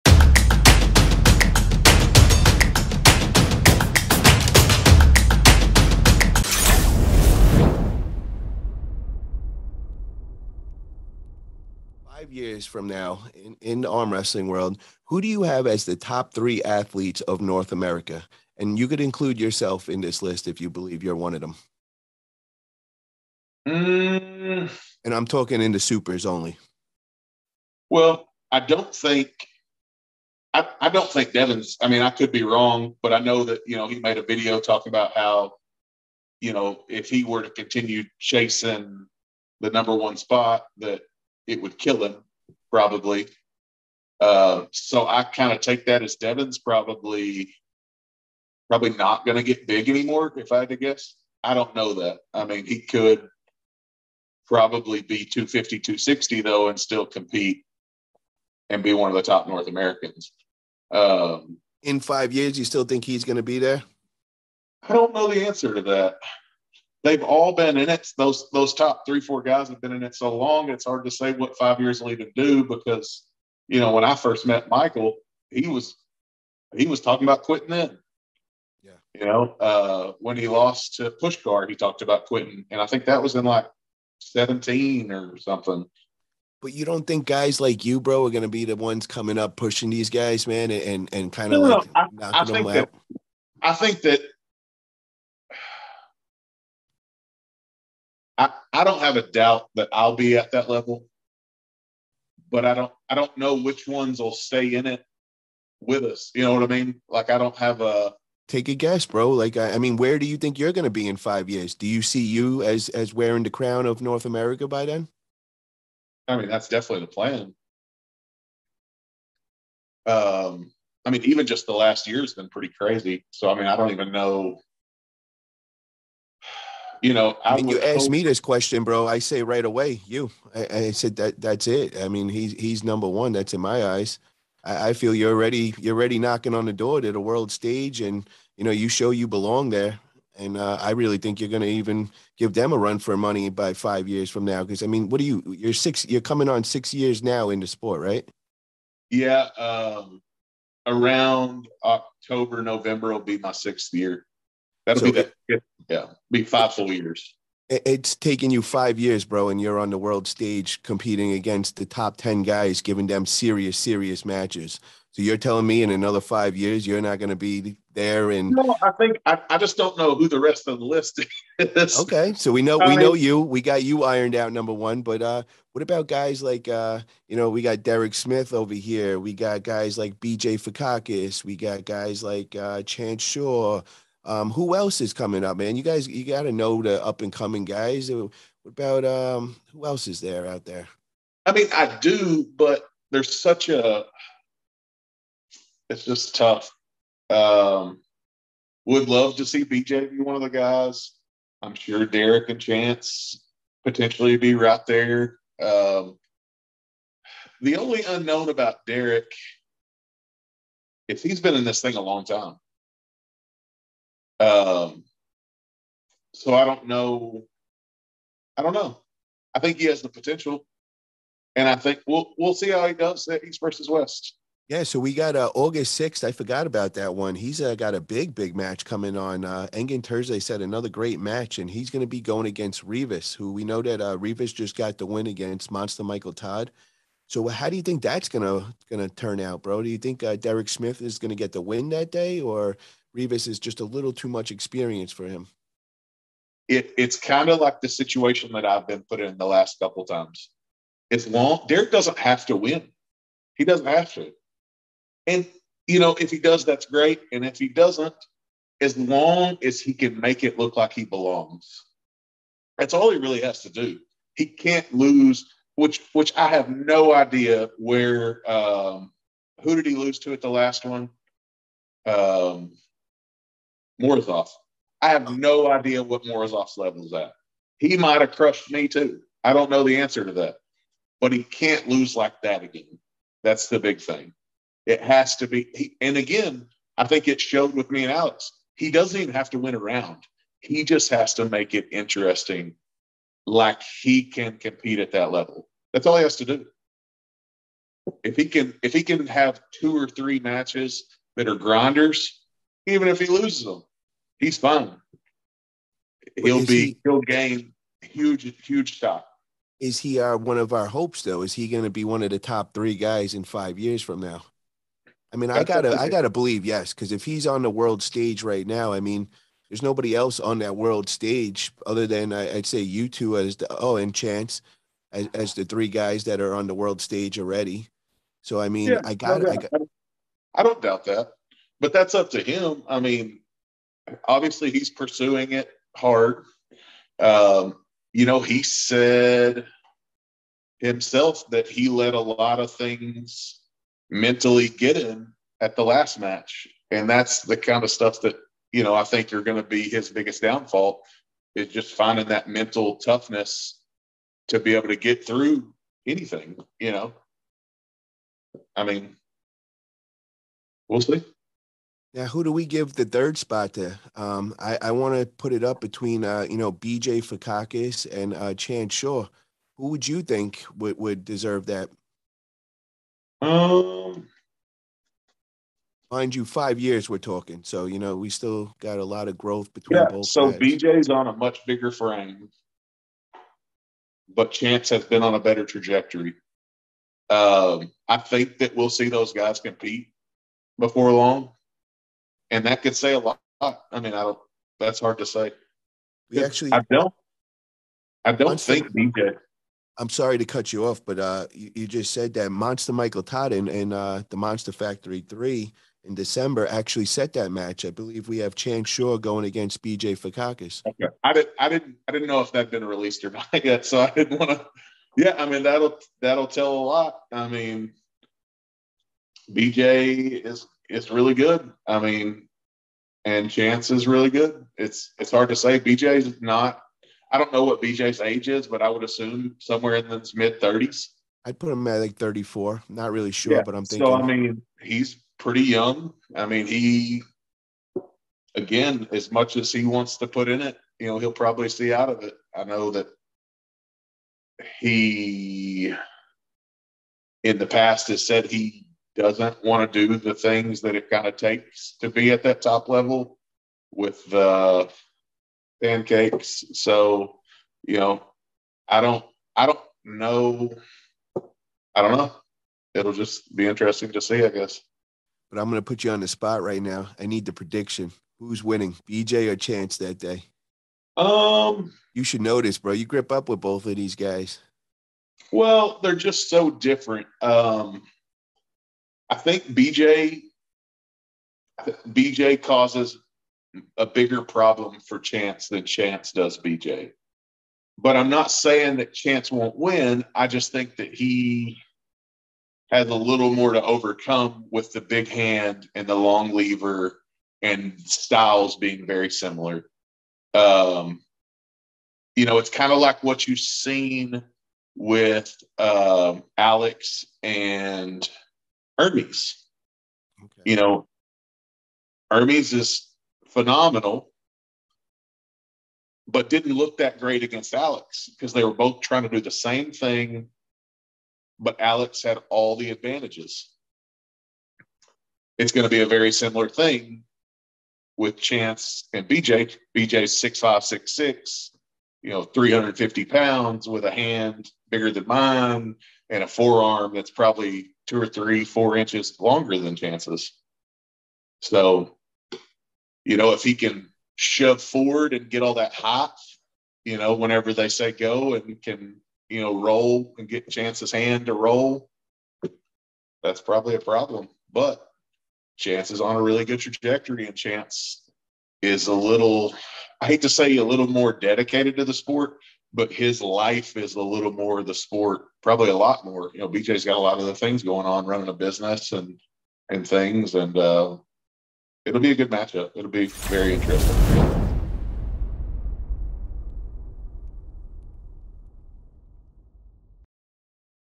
Corey West’s Top 3 In Five Years
Paul italia interviews Corey West And They Talk About Who He Would Have As The Top 3 Armwrestlers In 5 Years Time.